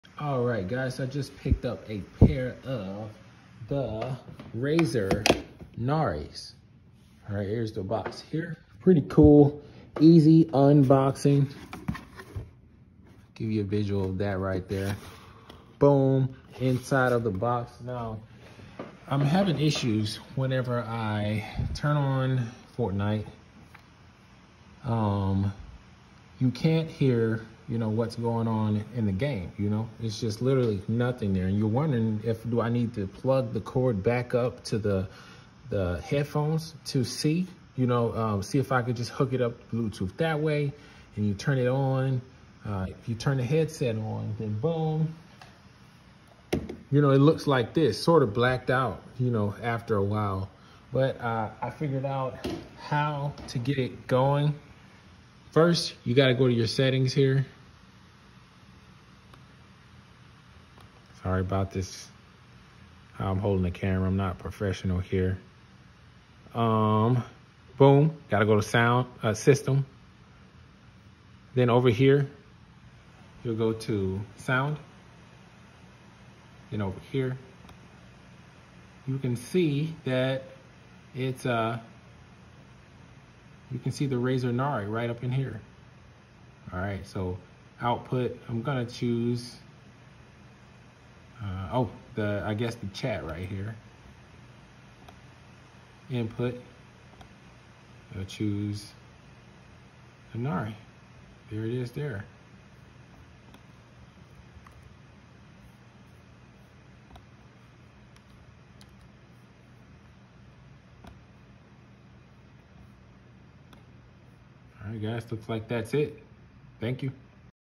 Razer Nari in Game No Sound fix
In this video I show you a quick fix to having no sound in game on PC.